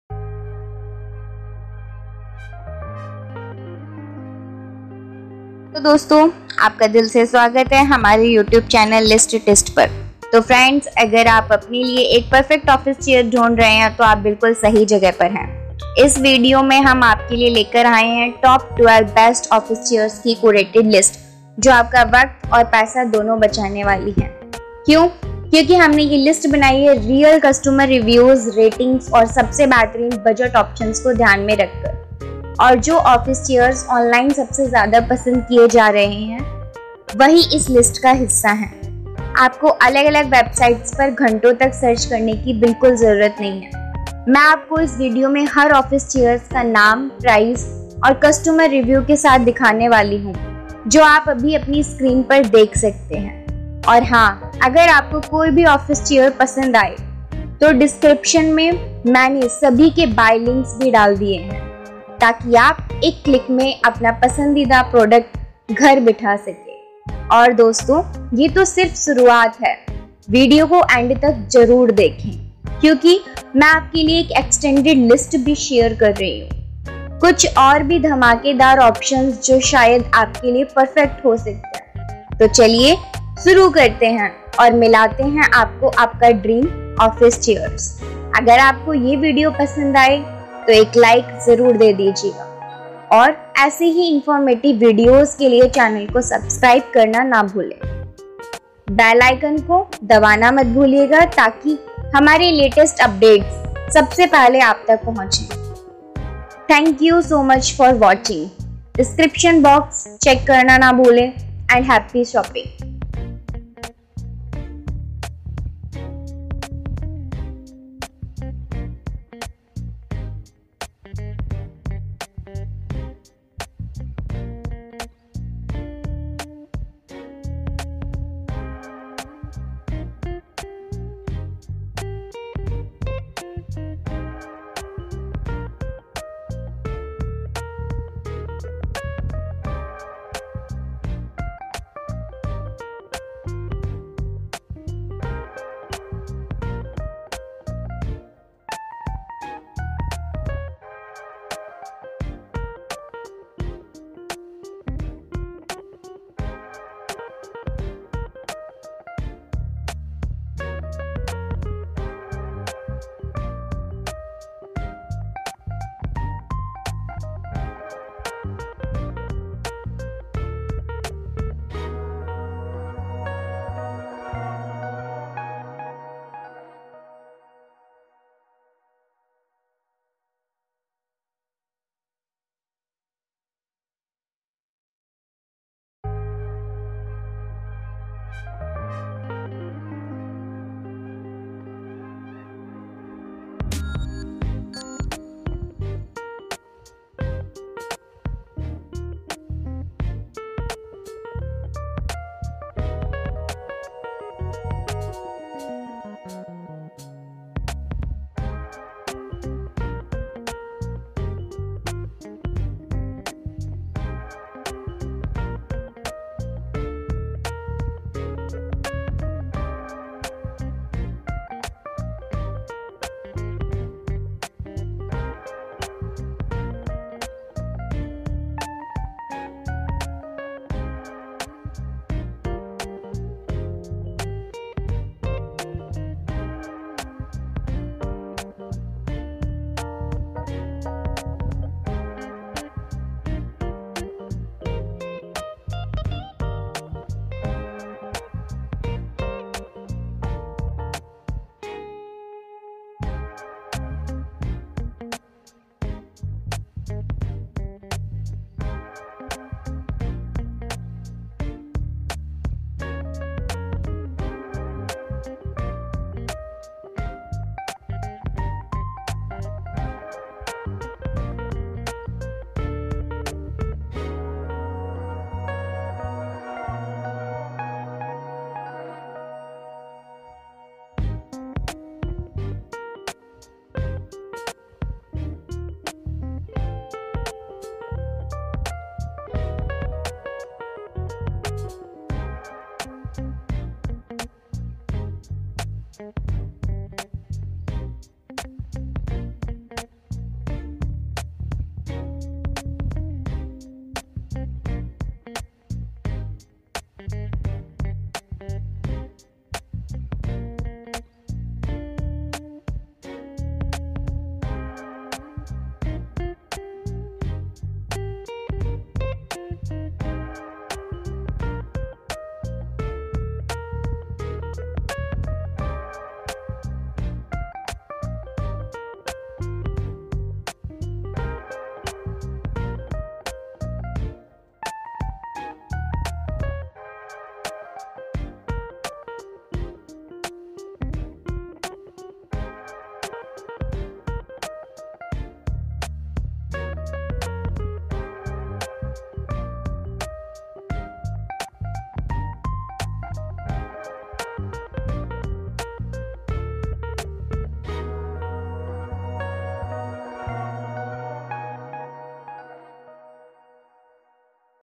Top 12 Best Office Chairs for Every Workspace with Ultimate Comfort in India 2025
Here is the List of 12 Best Office Chairs

ChairsTM Apollo Chrome Engineered Plastic

 TIMBER CHEESE Ergonomic REVOLVING Warranty

 30 Inch Gas Cooktop with 6 Metal Knob, Dalxo 5 Italy Defendi Burner Gas

 URBAN Miller Medium revolving Office

 Apex AM 5002 Apollo Medium Office

 beAAtho JS SJ Revolving Office Chair

 Sunon Office Computer Support Adjustable

Thanks
Admin of TISTLIST